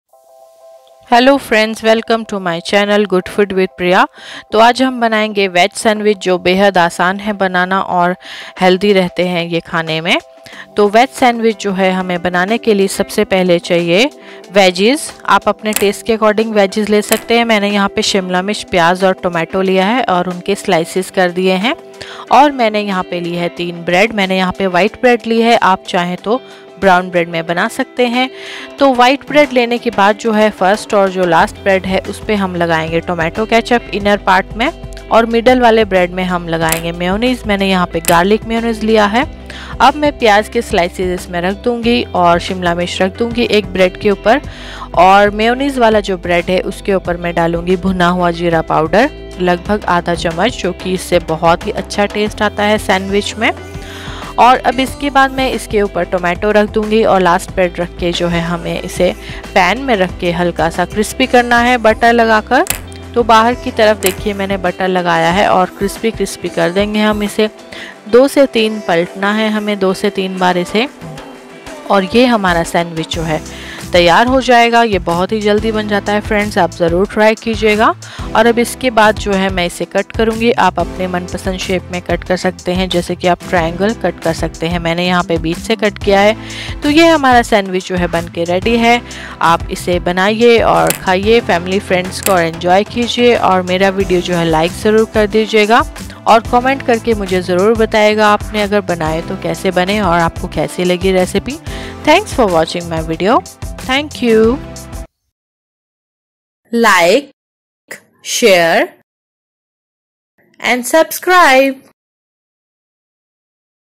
Triple layer Veg. Sandwich Within few minutes, Good food recipes With Priya.
Be safe from Covid .. You can by Mask at following link